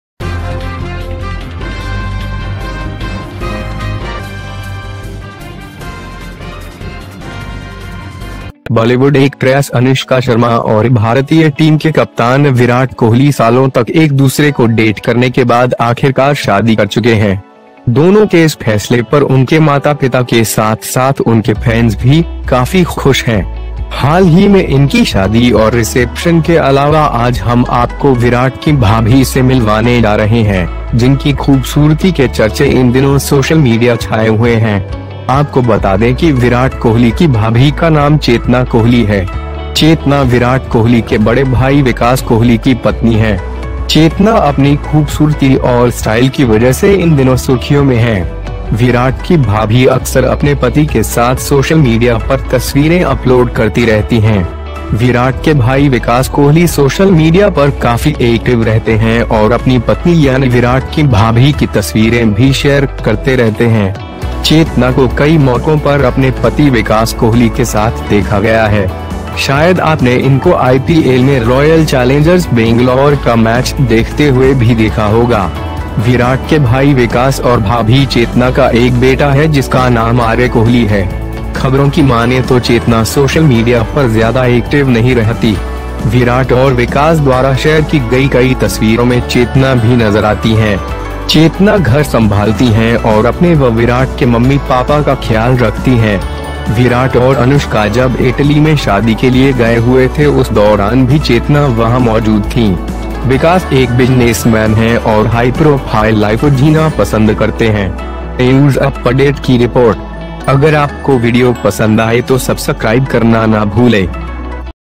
पत्नी अनुष्का से भी सुंदर हैं विराट कोहली की भाभी, देखकर आप भी हो जाएंगे दीवाने
Bollywood actress Anushka Sharma and Indian team captain Virat Kohli have finally married each other for a couple of years. Both of them have been happy with their parents as well as their fences.

Recently both of them gave a fabulous reception party to fabulous friends in Mumbai and Delhi. Whose pictures we shared with you. The news of marriage, honeymoon and reception remained in the media throughout the month. It can be said that Virat and Anushka's marriage are the biggest news of this year. Now this couple has gone to South Africa, where the Indian team has to play the match.

In addition to their marriage and reception recently, today we are going to introduce you to Virat's sister-in-law, whose beauty discussions are social media these days. Let us know that Virat Kohli's sister-in-law's name is Chetana Kohli. Chetana is Virat Kohli's elder brother Vikas Kohli's wife Consciousness is headlines these days due to its beauty and style.

Virat's sister-in-law often keeps uploading photos to social media with her husband. Virat's brother Vikas Kohli is very active on social media and also shares his wife's pictures of his sister-in-law. Consciousness has been seen on several occasions with her husband Vikas Kohli. Perhaps you have seen them in the IPL while watching the match of the Royal Challengers Bangalore. Virat's brother is a son of development and brother-in-law, whose name is Arya Kohli.

Considering the news, consciousness is no more active on social media. Consciousness also appears in many pictures shared by Virat and Vikas. Consciousness handles the house and the mother of her and the devotee takes care of Papa. When Virat and Anushka were in Italy for marriage, consciousness was also present there. Vikas is a businessman and likes to live high-profile life.